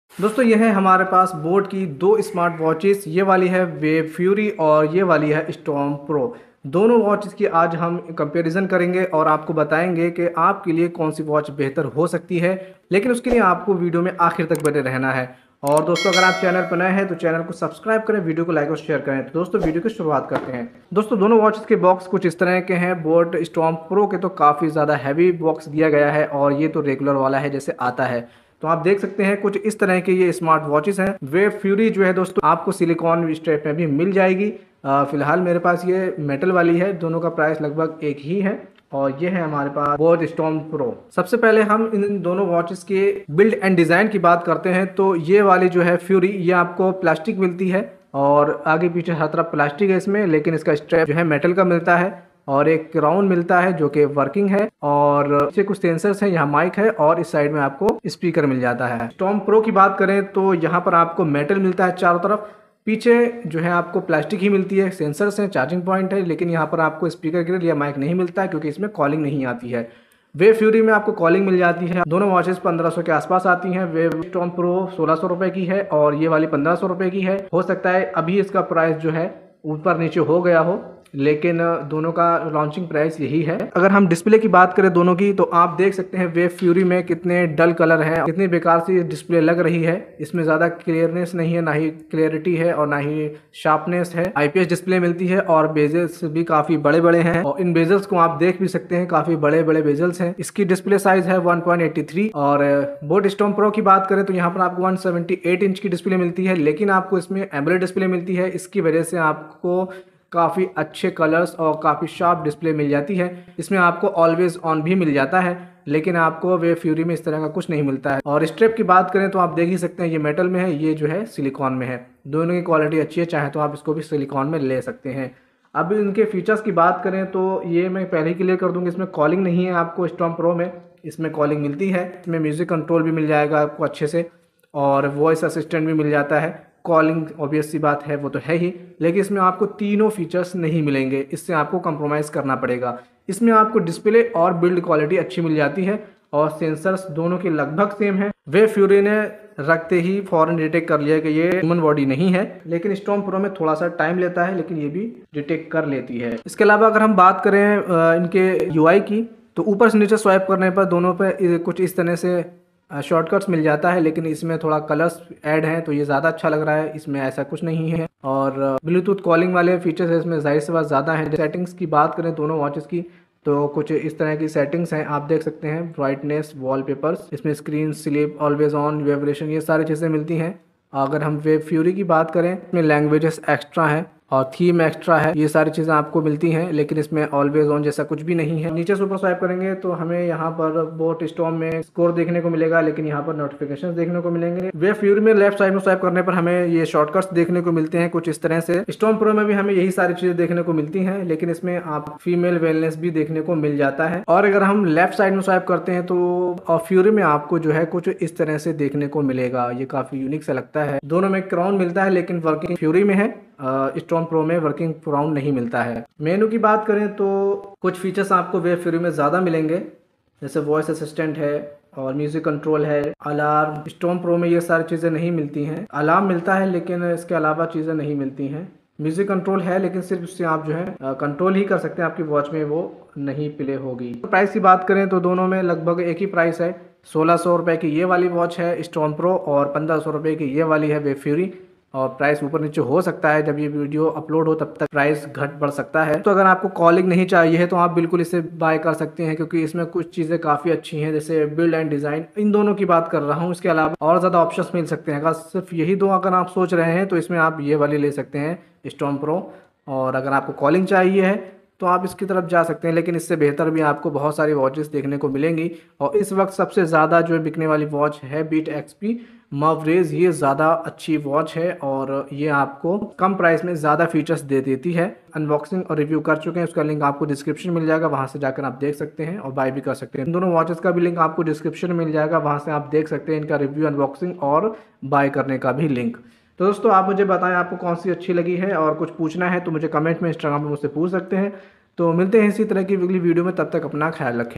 Boat Storm Pro vs Boat Wave Fury smartwatches | Best smartwatch under 1500
Hello Guys...
Is video me maine *Boat wave fury* smartwatch aur Boat Storm Pro smartwatche ko compare kia hai. Ye smartwatch kafi Kam price zada features le kar ayi hai.

Buy Link 🖇️

Boat wave fury

Boat Storm Pro

Boya by m1 mic

 DIGITEK DTR 480LW Portable Lightweight Aluminum Tripod with Operating Height 4.75 Feet Tripod